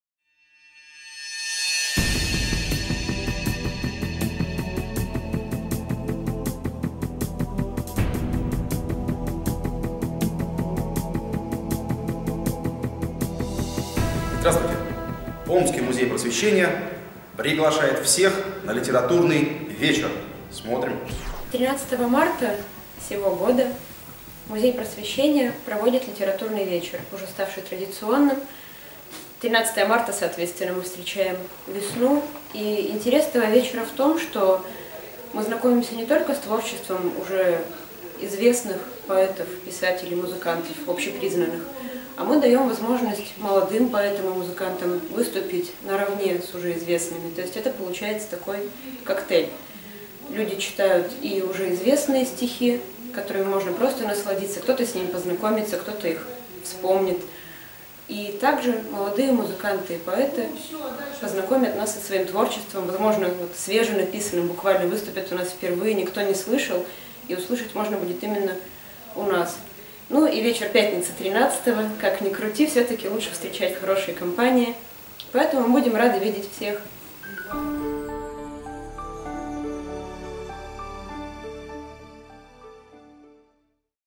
Новости Омских Музеев выпуск 25
"Март на пятки наступает/ Прогоняя зиму прочь / Днём снежок немножко тает / Подмораживает ночь". Омский музей просвещения приглашает встретить пятницу 13-го марта 2015 г. стихами омских поэтов. Приходите на литературный вечер!